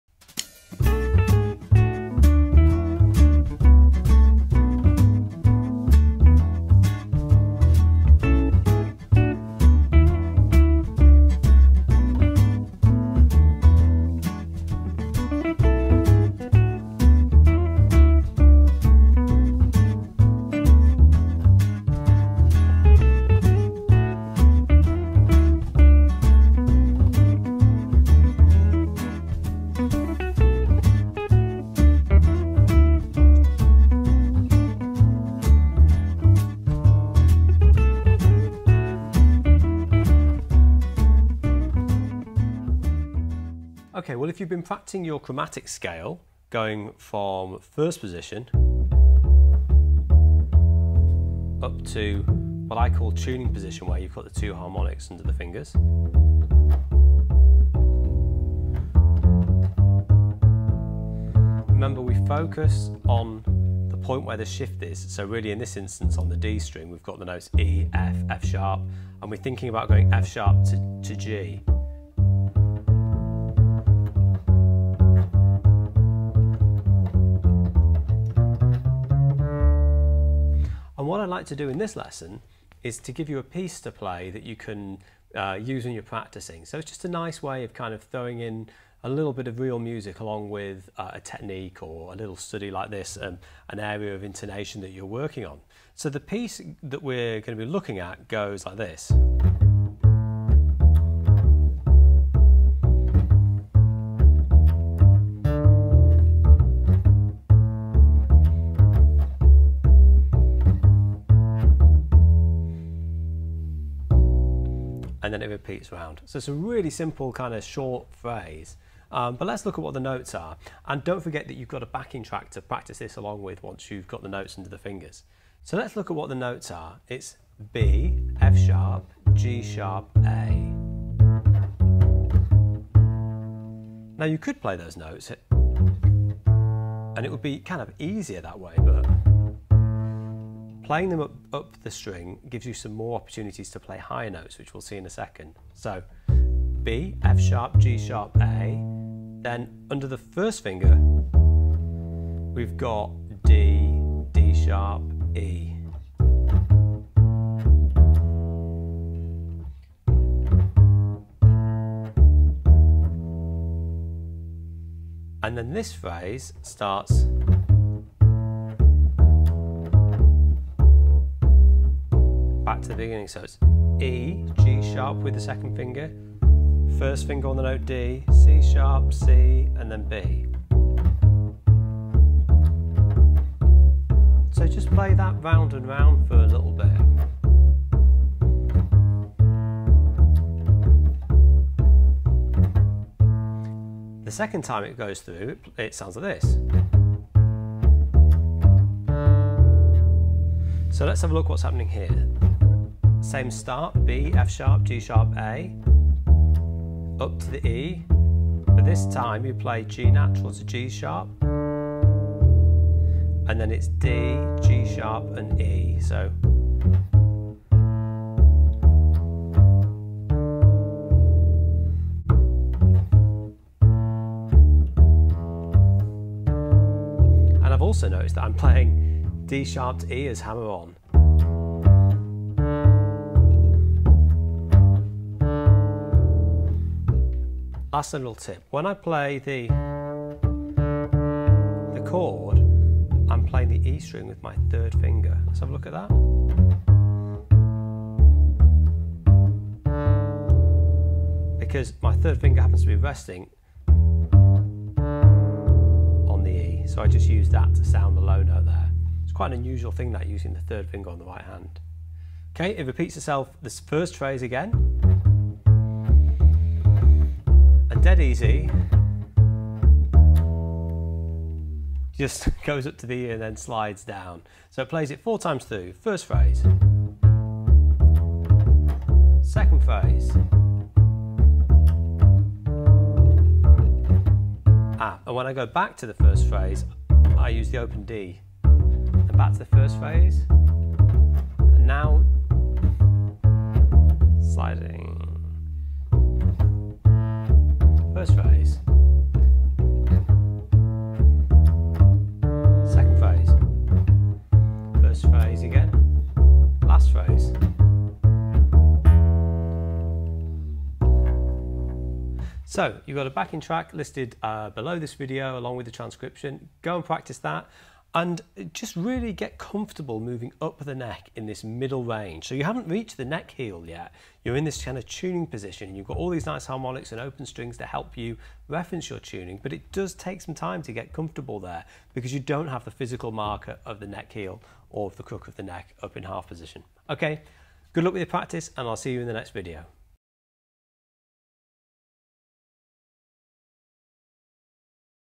Beginner's Double Bass Riff (1st position to tuning position)
In this video, I share a preview from my course 'Beginner's Double Bass'. It teaches a short riff to help you practice the chromatic scale. It moves between 1st position and tuning position and is a fun way of practising the chromatic scale. You can download the transcription of the riff and the backing track for free through this

If you enjoy the video please check out the full 6-hour course.